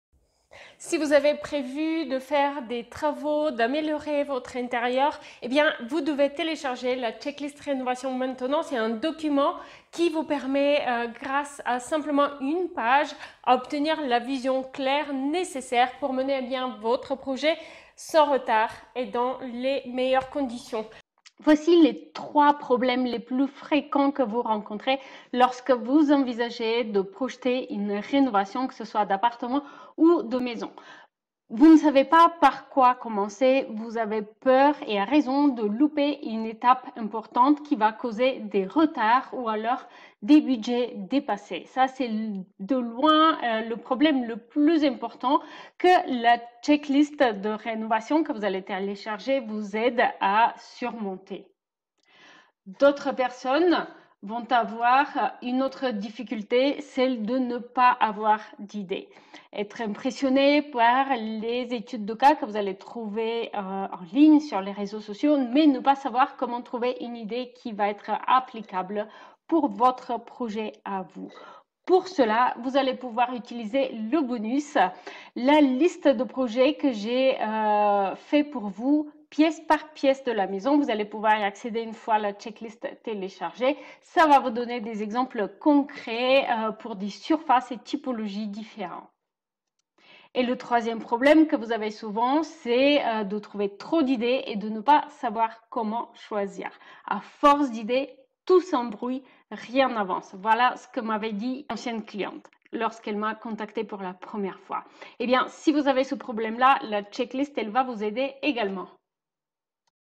Check-list des travaux de rénovation Une vision claire réno sur 1 page : PDF gratuit
Téléchargez la Checklist rénovation

Téléchargez maintenant votre copie de la check-list des travaux de rénovation offerte sur mon site web !

Une vision claire sur 1 page, voici le principe de cette check-list des travaux de rénovation ! Elle a déjà été téléchargée par plus de 10 000 personnes.

En bonus, vous allez accéder à la liste de projets récents, avec une fiche à télécharger pour chaque fiche ! De quoi trouver une inspiration pertinente pour voter projet!

Vous allez pouvoir dépasser 3 limitations:

- par quoi commencer pour mener à bien son projet de rénovation sans retards et en respectant son budget
- comment trouver des idées pertinentes
- a force d'idée tout s'embrouille, rien n'avance- comment choisir